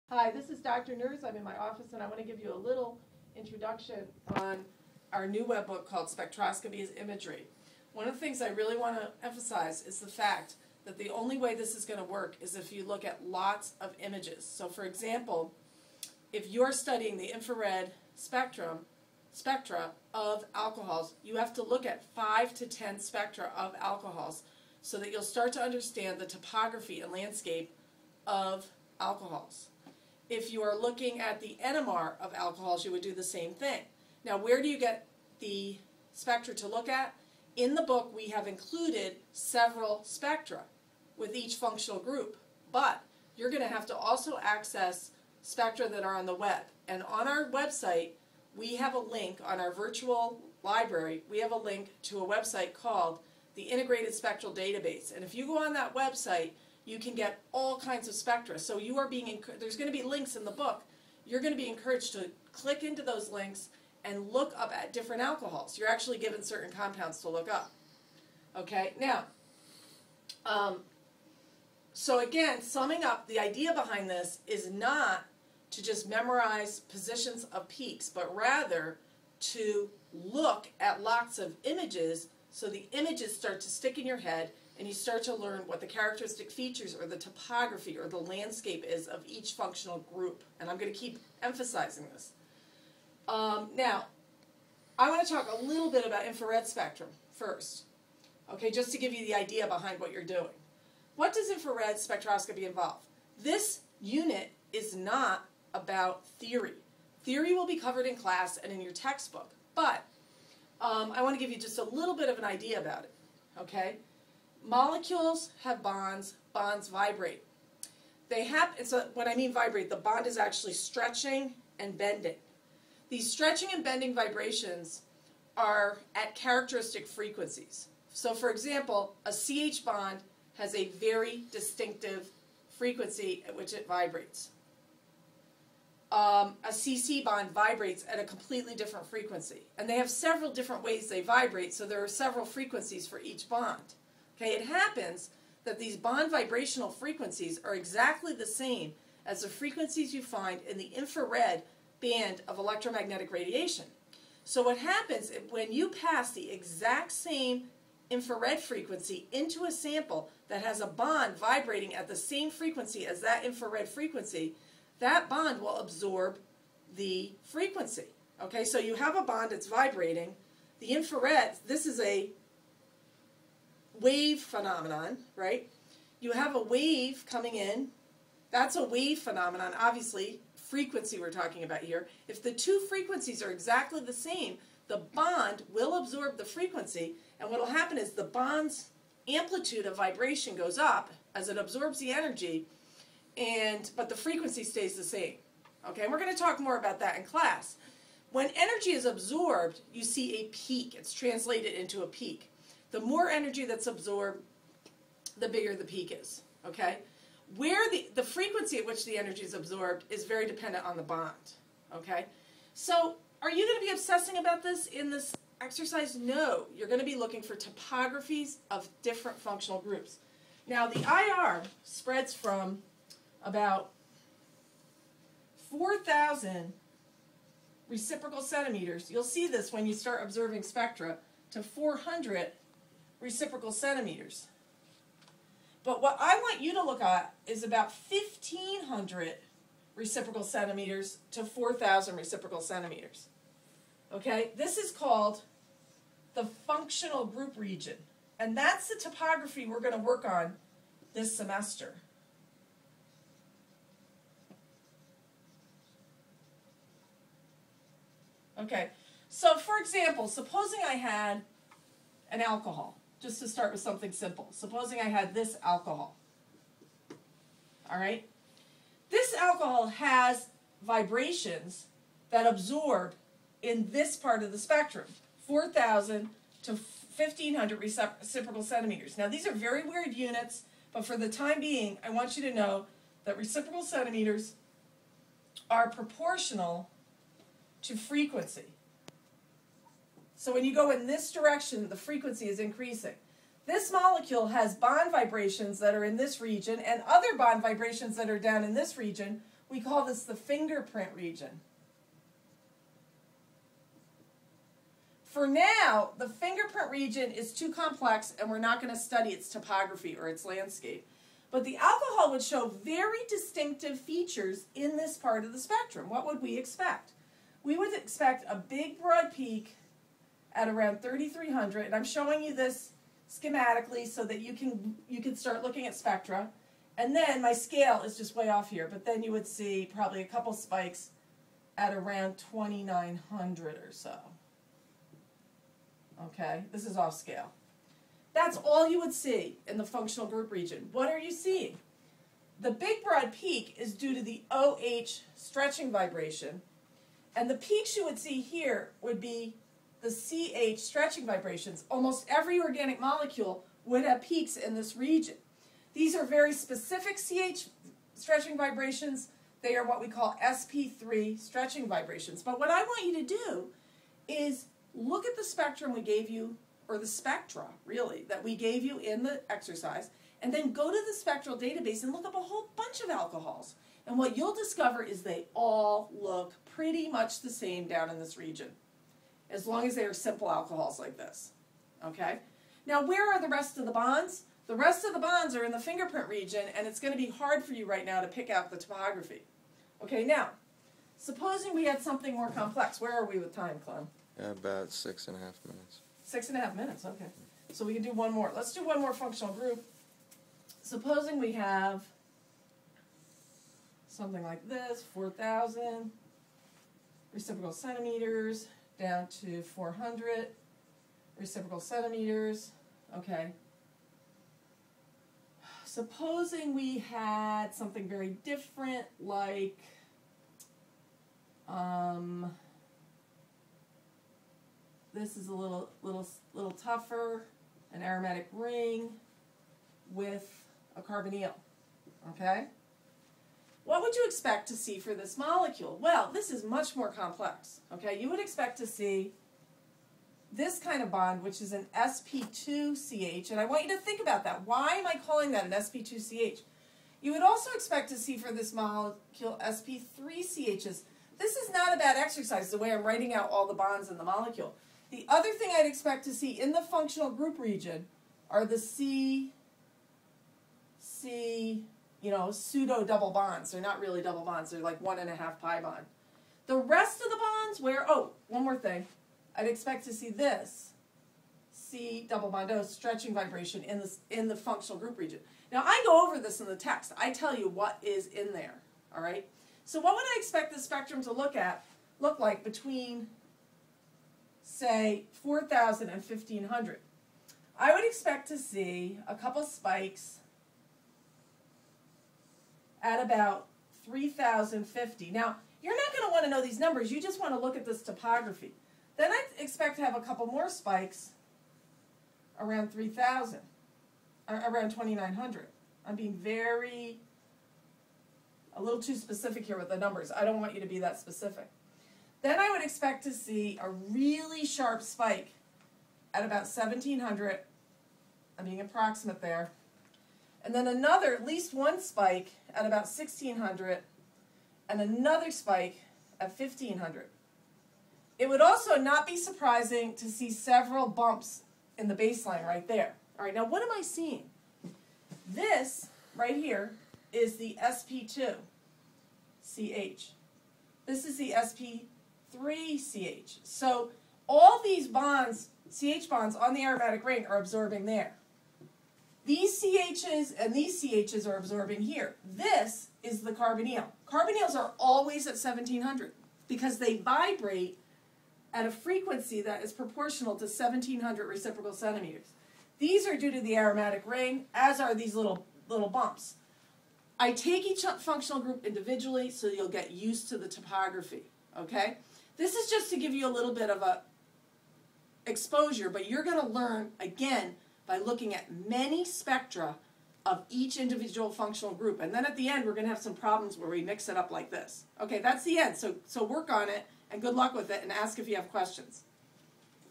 Spectroscopy as Imagery Part I Introduction and Infrared Basics
Dr. Nerz at Bryn Mawr gives the background for the Spectroscopy as Imaging Webbook.